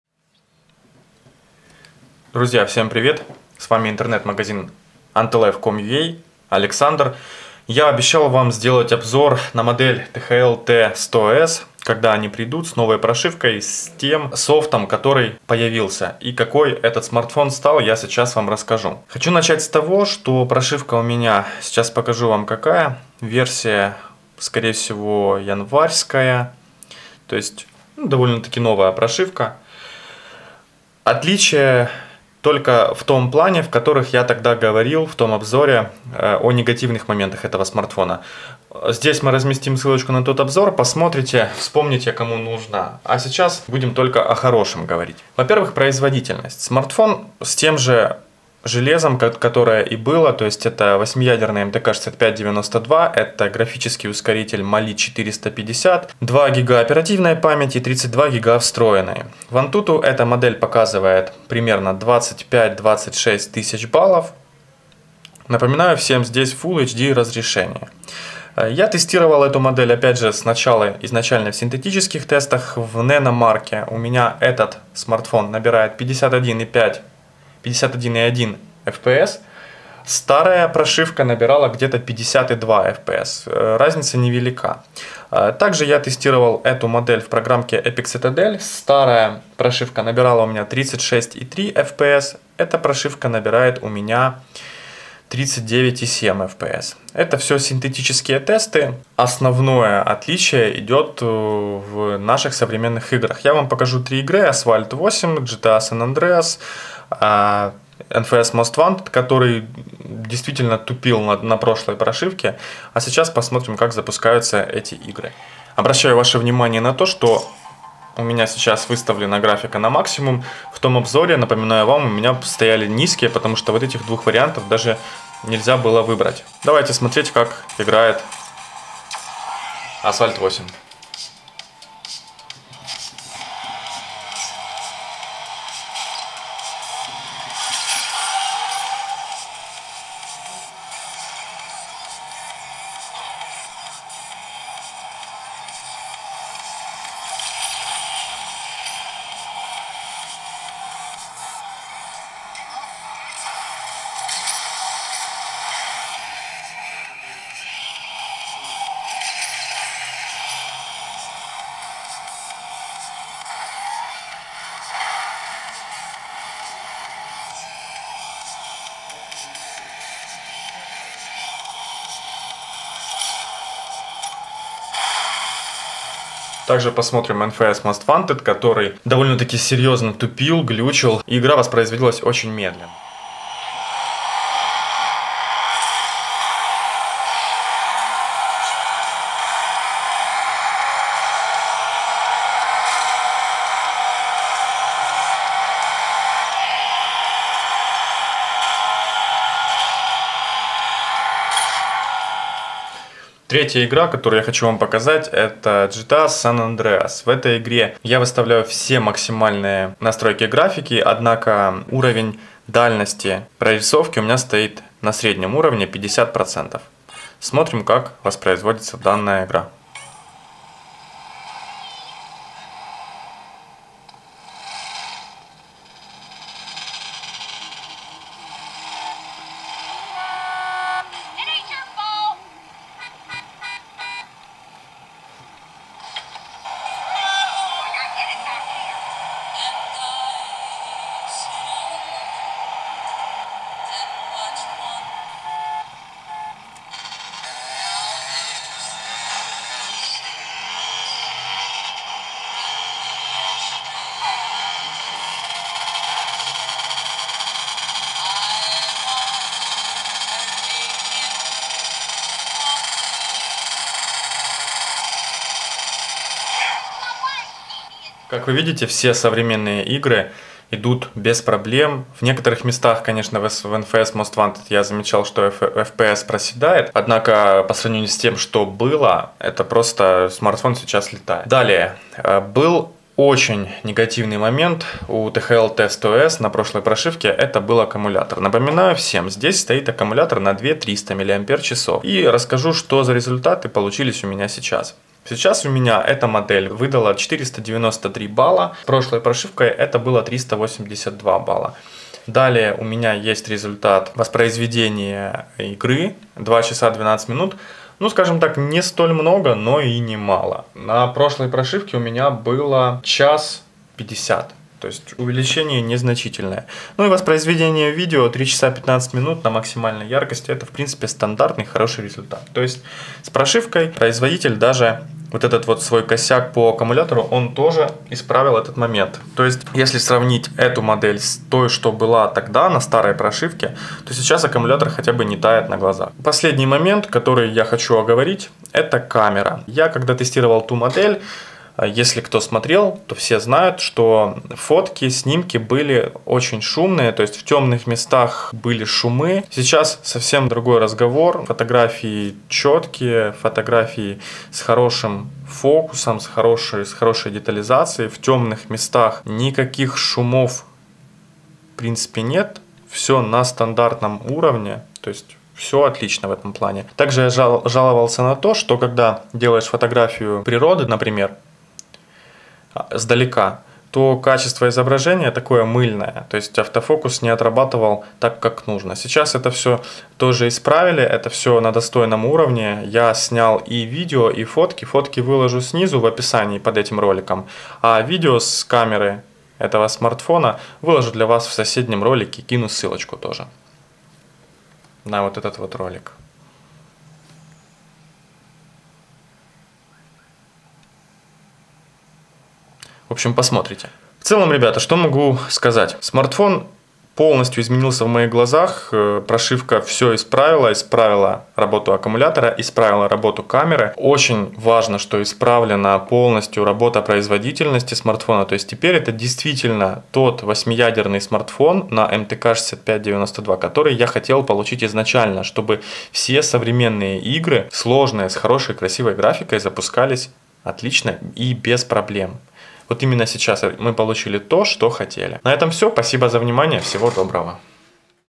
THL T100S MTK6592 обновленная прошивка, разница!
Смартфон THL T100S начал себя вести по другому. Игры начали воспроизводиться с максимальной графикой и без глюков, производительности достаточно для любой игры. Также могу сказать, что и аккумулятор начал работать, как минимум, так, как должен. Камера перестала шуметь, темные объекты просто темные, а не шумные.